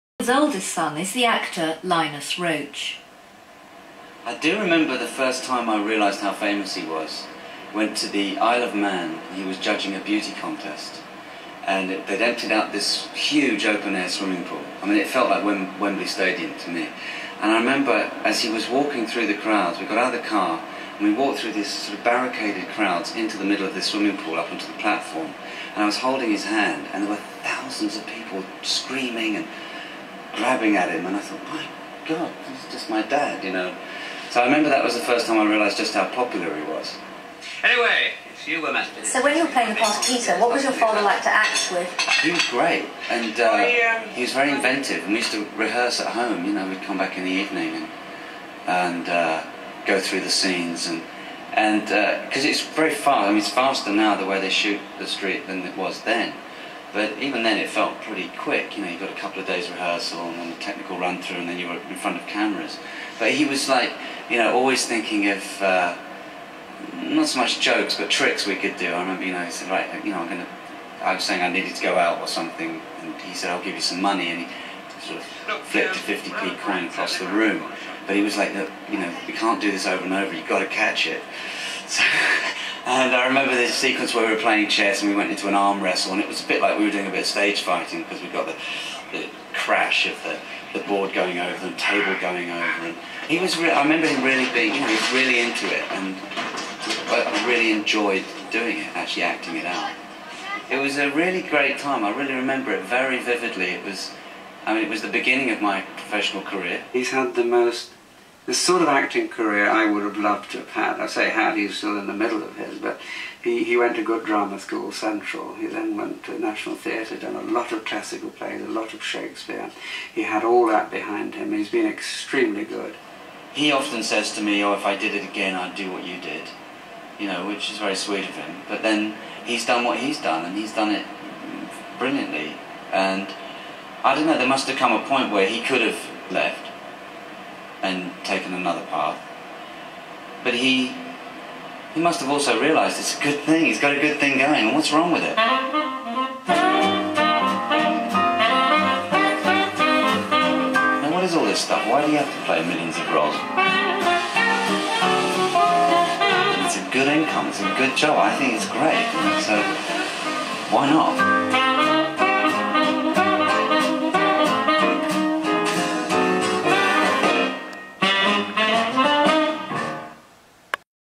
Linus Roache reflects on Coronation Street
Linus Roache speaking about his father William and acting on Coronation Street as a boy.